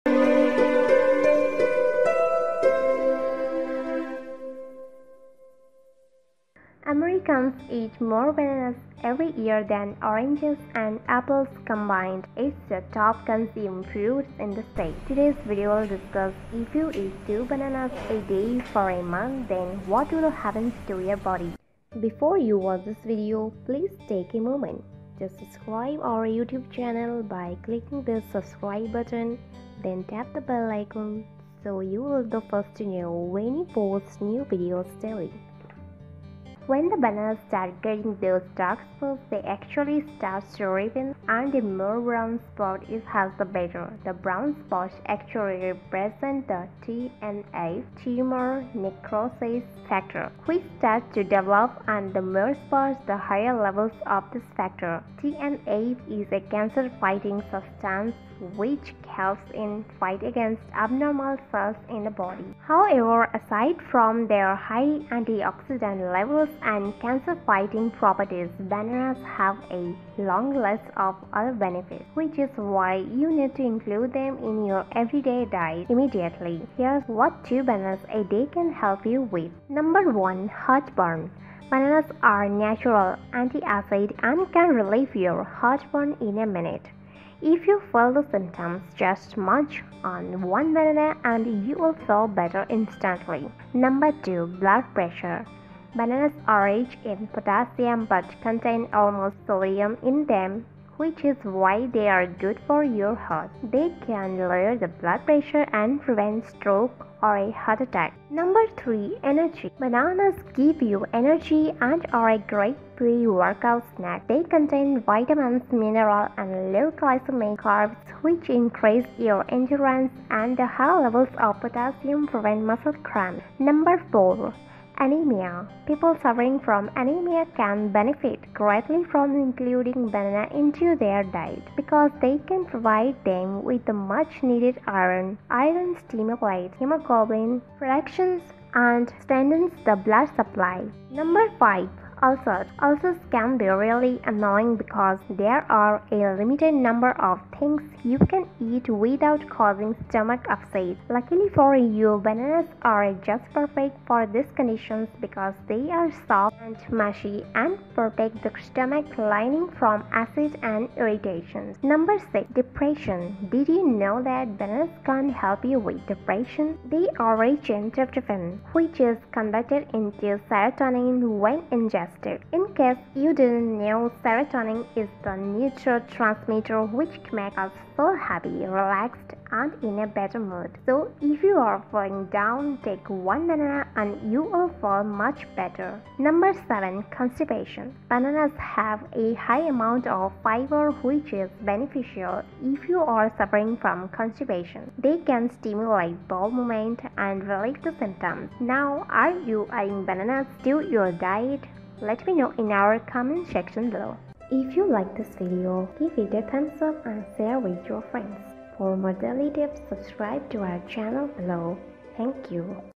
If You Eat 2 Bananas Per Day For A Month, This Is What Happens To Your Body - Remedies One
Americans eat a bigger number of bananas consistently than oranges and apples joined; it's the top devoured organic product in the states.

The present video will talk about on the off chance that you Eat 2 Bananas per Day for a Month, at that point what Will Befall Your Body!

At the point when the bananas begin getting those dim spots they really begin to mature and brown should spots it has as much as possible. The dark colored spots really speak to the TNF, (tumor putrefaction factor) which begins to create and the more spots the larger amounts of this factor. TNF is a malignant growth battling substance which aides in the battle against unusual cells in the body.

Be that as it may, beside their high cell reinforcement levels and disease battling properties bananas have a not insignificant rundown of different advantages which is the reason you have to incorporate them in your ordinary eating regimen right away. This is what two bananas daily can assist you with

1) Heartburn – Bananas are normal enemy of corrosive and can soothe your indigestion in a moment. On the off chance that you feel the indications simply crunch on one banana and you'll feel better in a flash.

2) Blood weight,

Bananas are wealthy in potassium however contain no sodium in them which is for what reason they're useful for your heart. They can bring down the circulatory strain and counteract stroke or a heart assault.

3) Energy,

Bananas give you vitality and are an incredible pre-exercise nibble. They contain nutrients, minerals and low glycemic carbs which increment your perseverance and the elevated amounts of potassium forestall muscle issues.

4) Anemia,

Individuals experiencing pallor can profit significantly from bringing bananas into their eating routine since they can furnish them with the genuinely necessary iron. Iron invigorates hemoglobin generation and reinforces the blood supply.

Presently, would you say you are adding bananas to your eating regimen? Tell me in our remark area underneath.